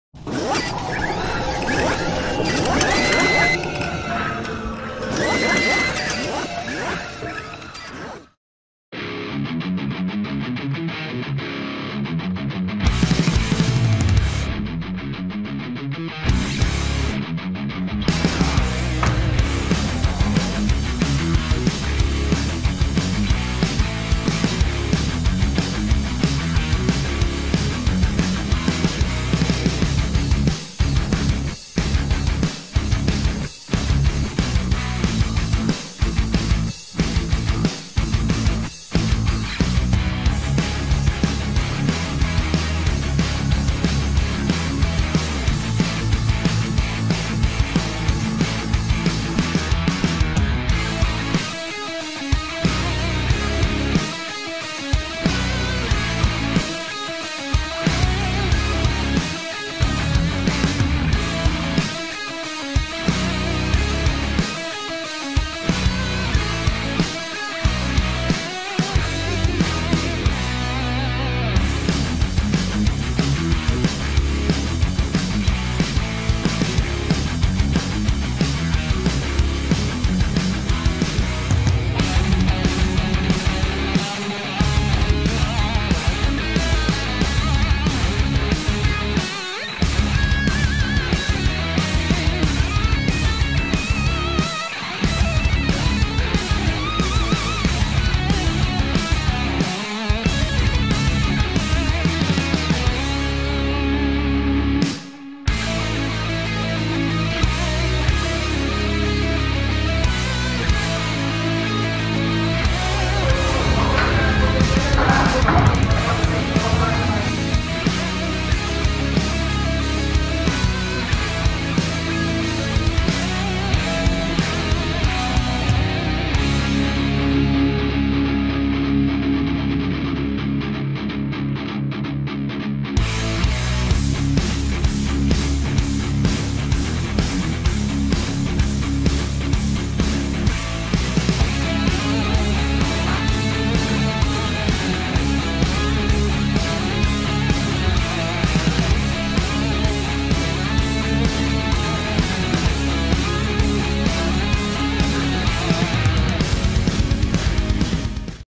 Igedz Back Workout
Program latihan persiapan Mr. Asia event yang akan digelar Oktober di Singapura. Atlet Binaraga Indonesia ini salah satu atlet yang akan turun dalam event tersebut, selain dari pemenang yang akan dipilih pada event I-BOM "Battle Of Mucle", 21 Mei 2017 @Grand Pramuka Square, Jakarta.

See u guys!